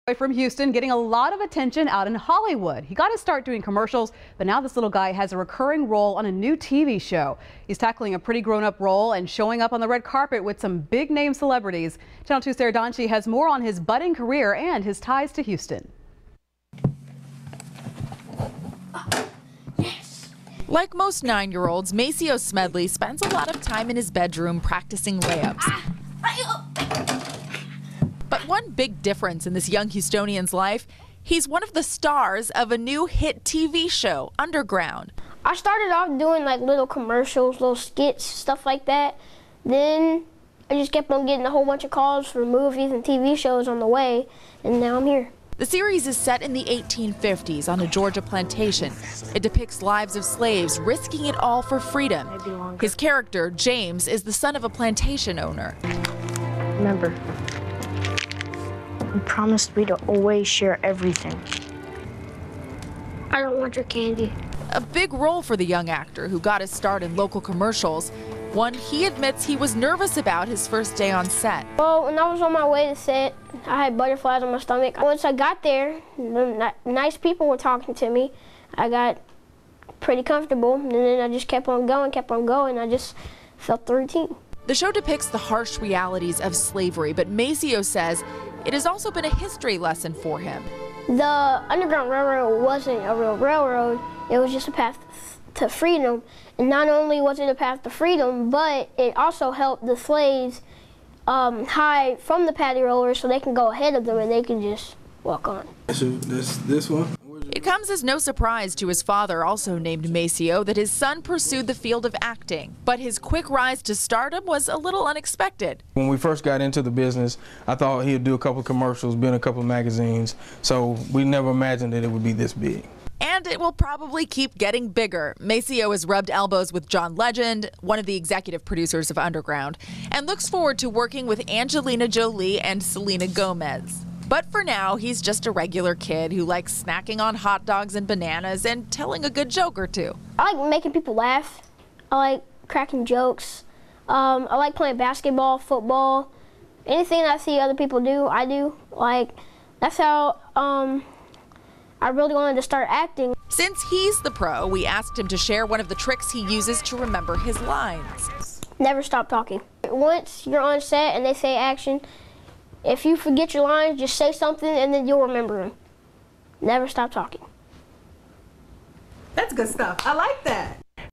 Houston boy makes it big in Hollywood
A boy from Houston is getting a lot of attention out in Hollywood.